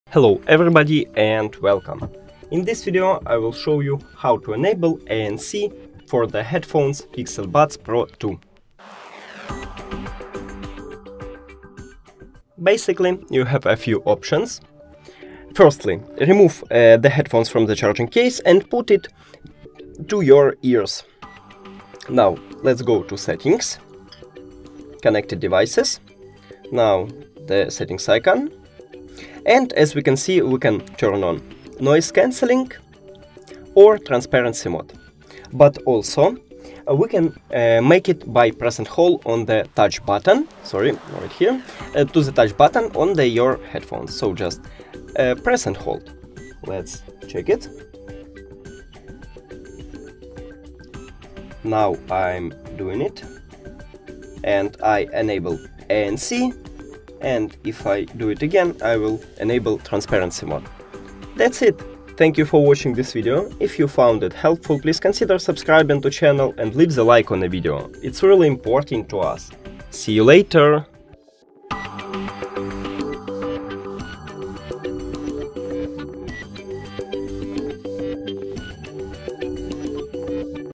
How to Enable Noise Cancelling on Google Pixel Buds Pro 2 | Optimize Your Listening Experience
In this tutorial, we’ll show you how to enable noise cancelling on your Google Pixel Buds Pro 2 to optimize your listening experience. Noise cancelling technology helps block out external sounds, allowing you to immerse yourself fully in your music, podcasts, or calls. We’ll provide step-by-step instructions to guide you through the process of activating this feature, ensuring you can enjoy uninterrupted audio wherever you are.

How to enable noise cancelling on Google Pixel Buds Pro 2?
What are the benefits of using noise cancelling for your listening experience?
How to customize noise cancellation settings on your Pixel Buds Pro 2?